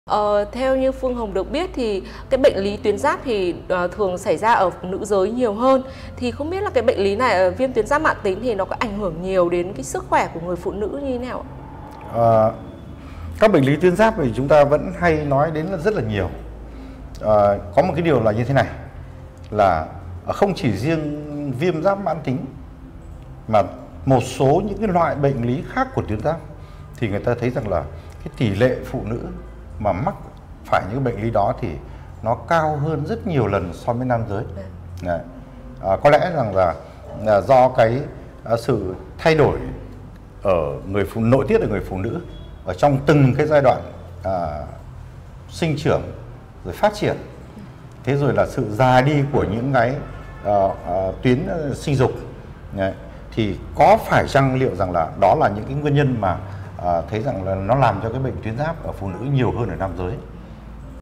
U bướu giáp nhân lành tính là gì? Vì sao tỷ lệ mắc bệnh ở nữ giới nhiều hơn nam?
Câu hỏi: Bệnh u bướu giáp nhân lành tính là gì? Có phải nữ mắc nhiều hơn nam không và biểu hiện của bệnh như thế nào?
▶ Tìm hiểu các bệnh ung thư cũng như các phương pháp điều trị tại
▶ Tham gia Kết nối ung thư Việt Nam để cùng chia sẻ các vấn đề bạn gặp phải